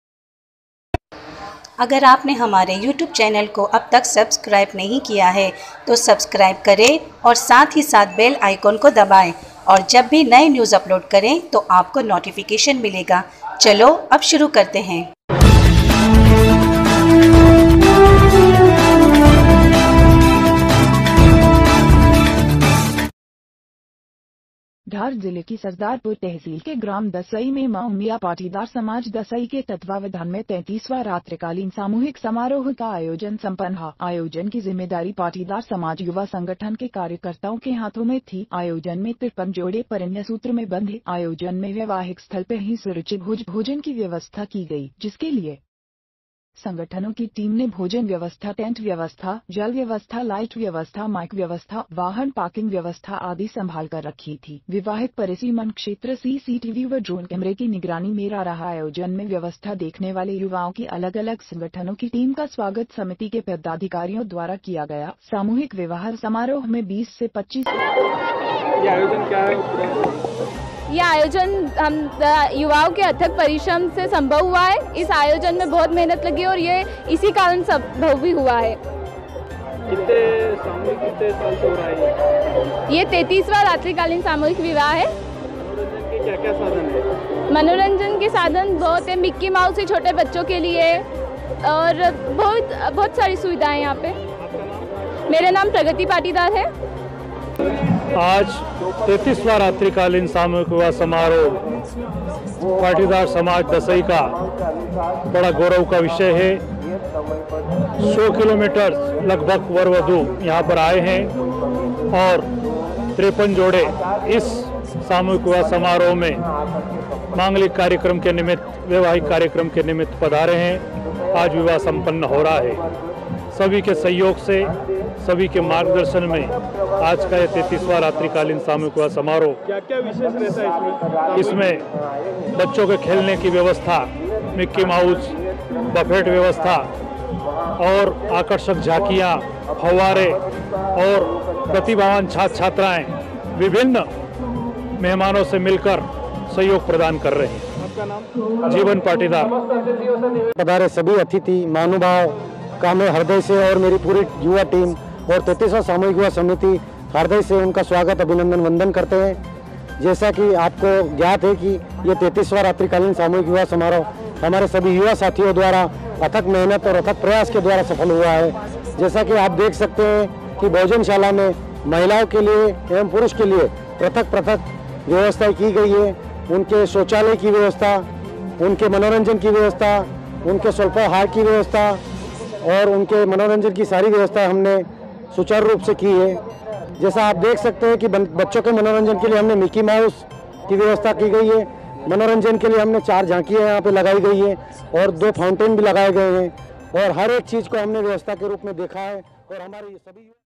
उमिया पाटीदार समाज दसई के तत्वावधान मे 33 वा रात्रिकालीन सामूहिक समारोह का आयोजन
Video from Abhay///kumar